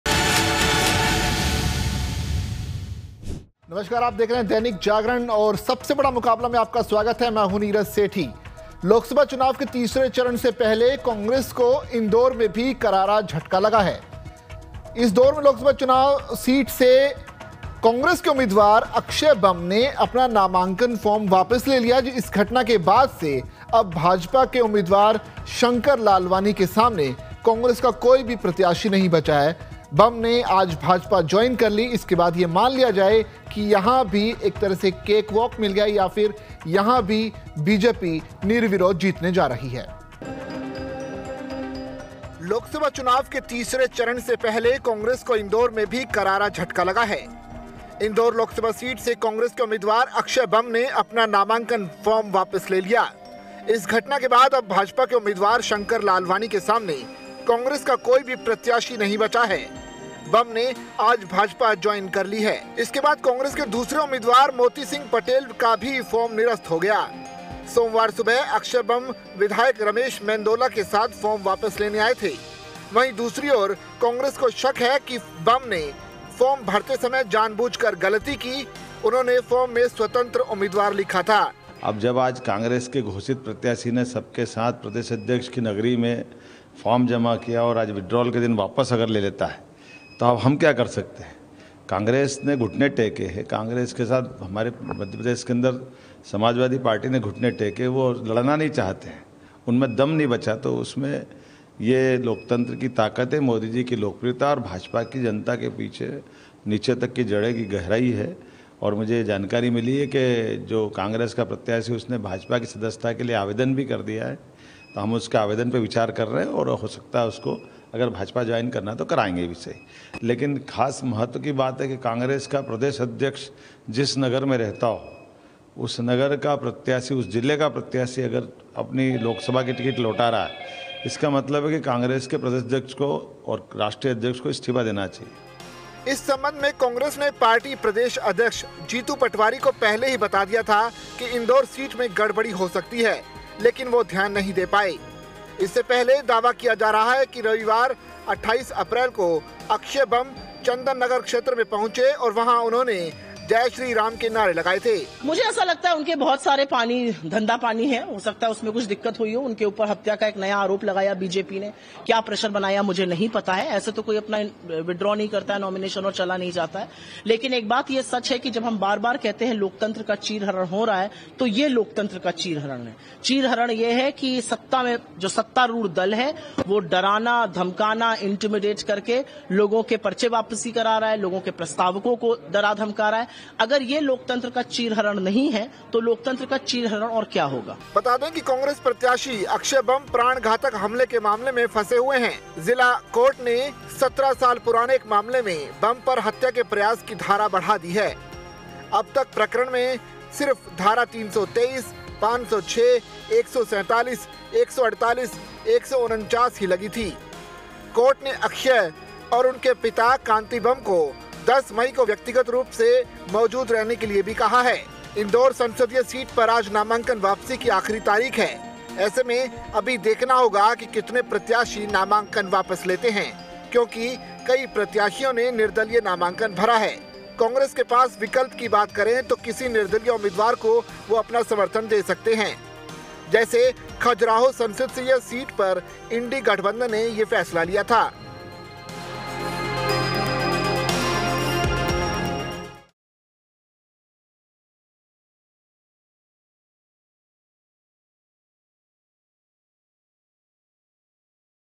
Loksabha Election 2024: इंदौर में Congress को झटका, Akshay Kanti Bam ने Nomination लिया वापस
लोकसभा चुनाव के तीसरे चरण से पहले कांग्रेस को इंदौर में भी करारा झटका लगा है. बता दें कि इंदौर लोकसभा सीट से कांग्रेस के उम्‍मीदवार अक्षय बम ने अपना नामांकन फॉर्म वापस ले लिया है. इस घटना के बाद अब भाजपा के उम्‍मीदवार शंकर लालवानी के सामने कांग्रेस का कोई भी प्रत्‍याशी नहीं बचा है. बम ने आज भाजपा ज्वॉइन कर ली है.